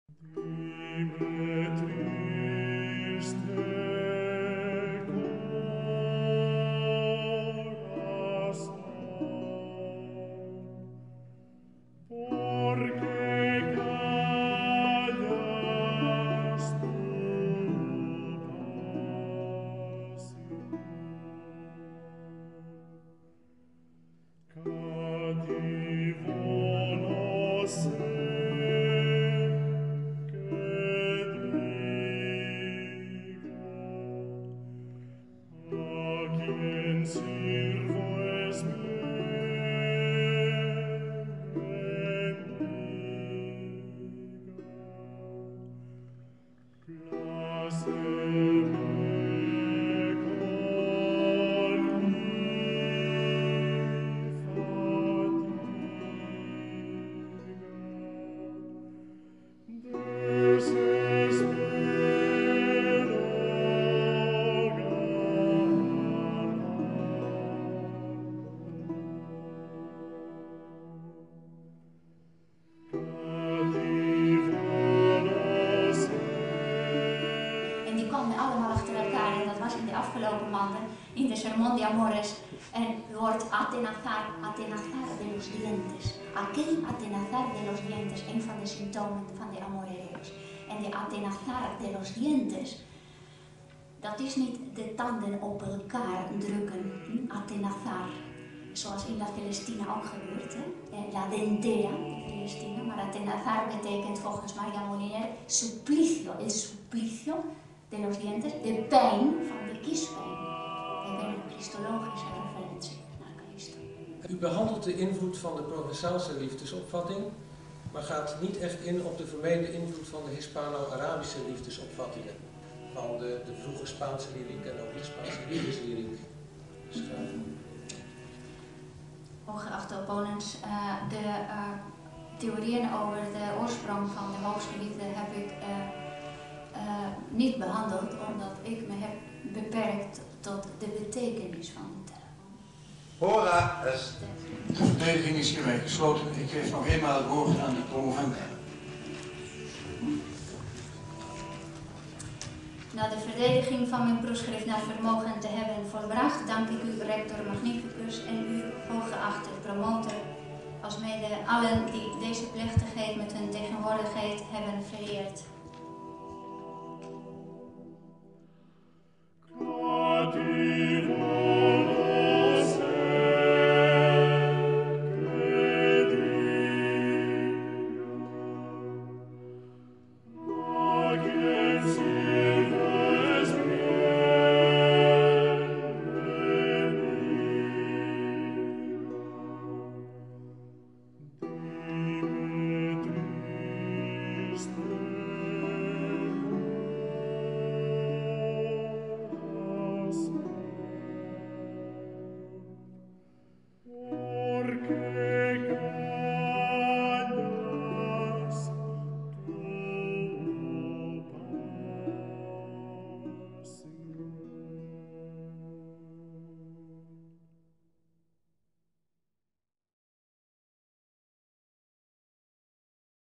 EN TOEN: I. M. Elena Carrillo
"Bron van leven en liefde", en onze vriendin, overleed